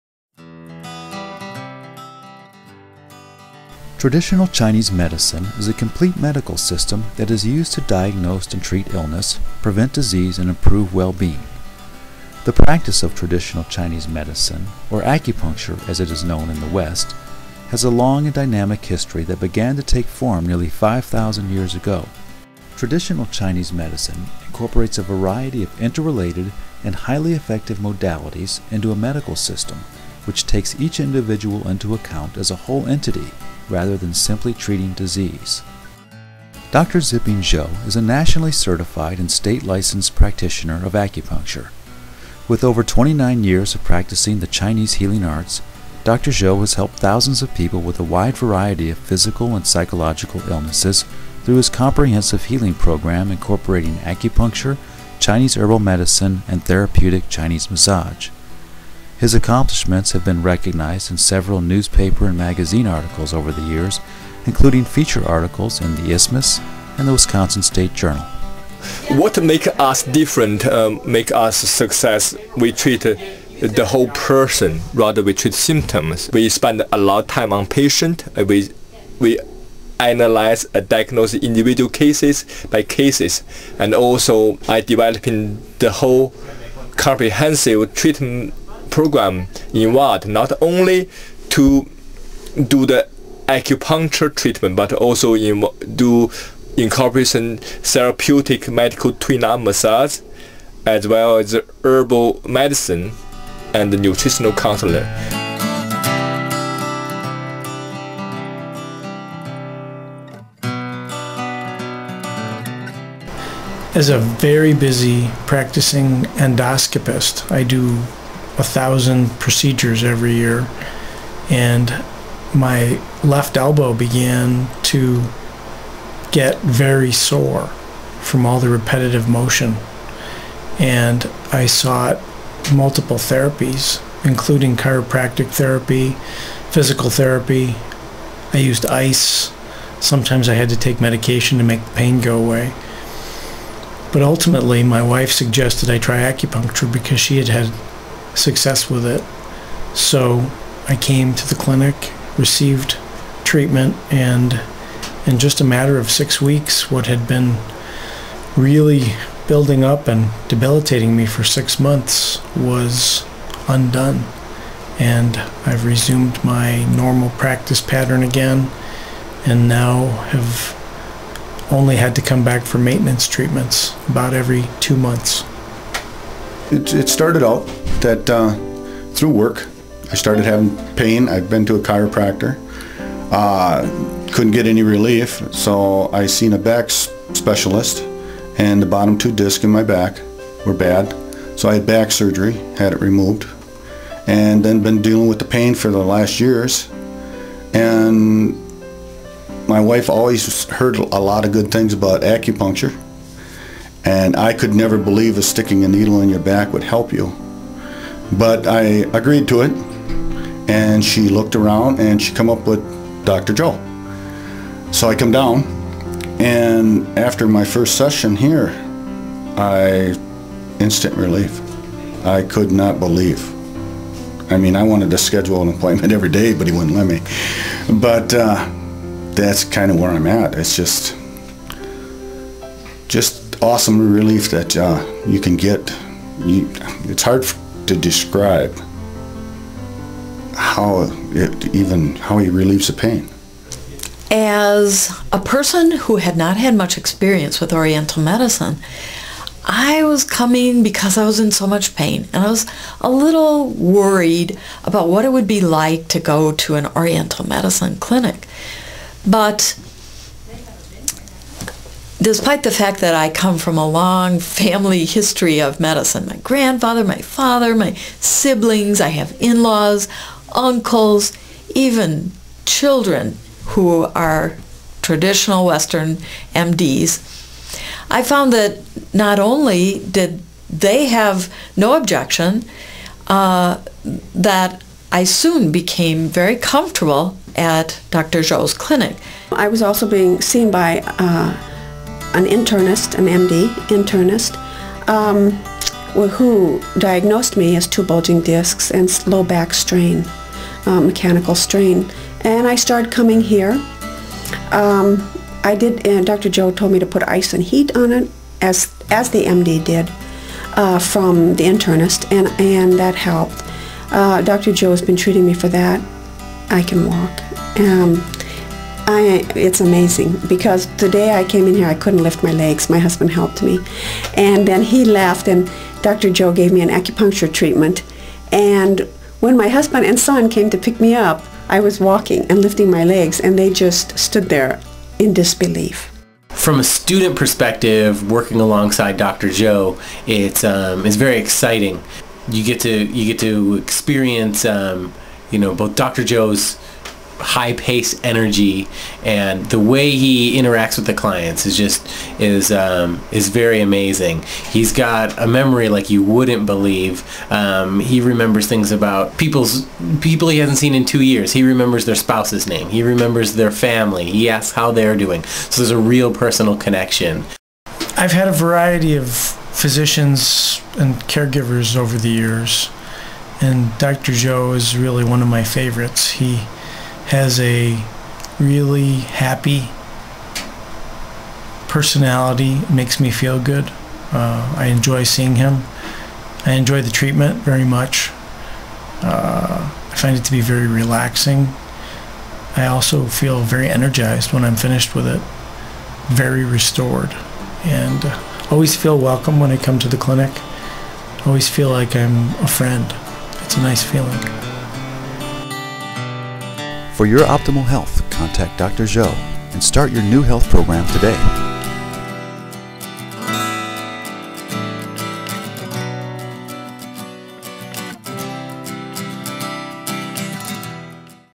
Dr. Zhou's Acupuncture and Herb Clinic
A informational video about Dr. Xiping Zhou's acupuncture clinic in Madison Wisconsin.